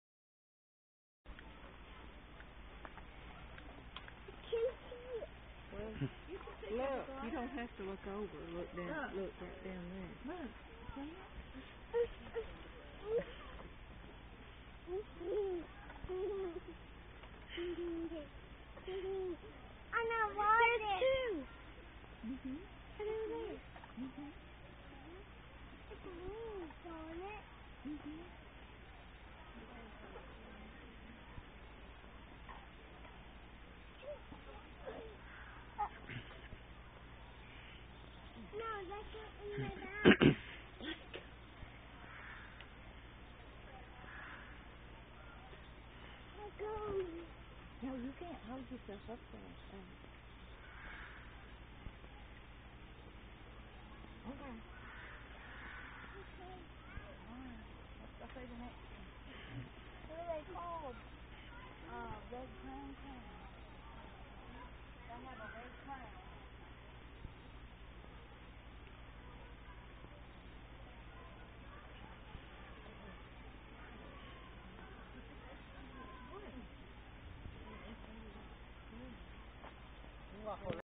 Red-Crowned Cranes (Nashville Zoo, 10-19-14)
Red-Crowned Cranes from the Nashville Zoo.

The picture quality's not the best, since it was recorded on my cell phone.